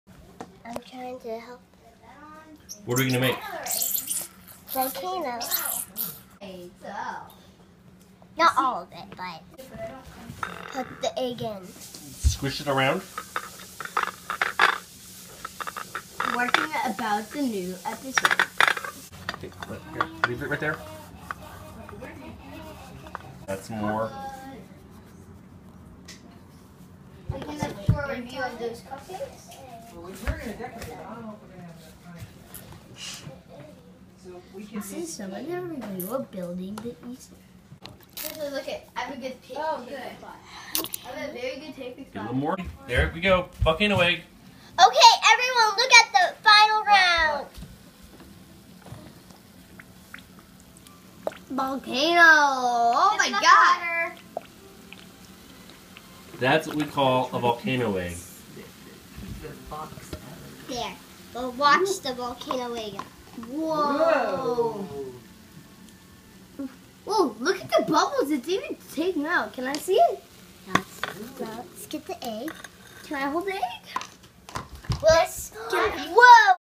Egg volcano!
A unique way of dying easter eggs. Volcano dying is fun and educational.